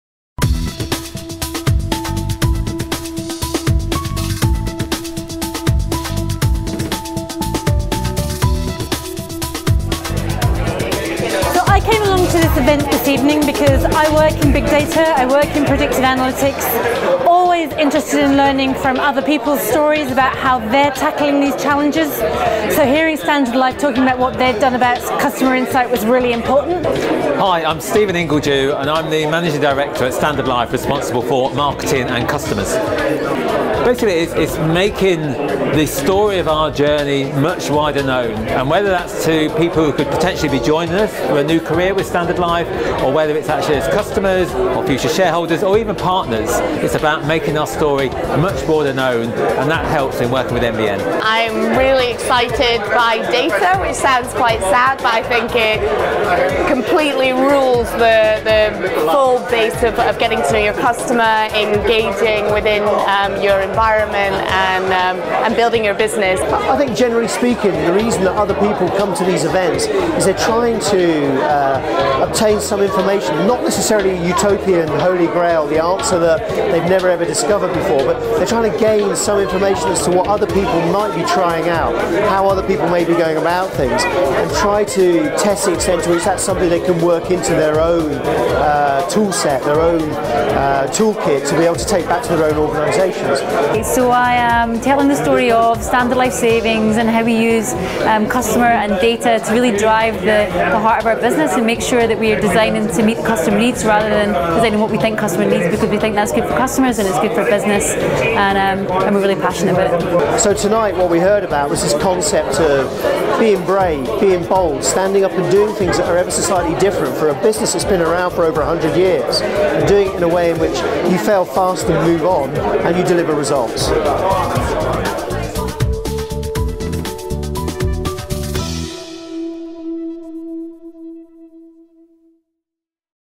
Standard Life - Discovering the true Customer Journey
26/8/15 - We held an event in the Standard Life headquarters in Edinburgh to hear about their journey using data analytics.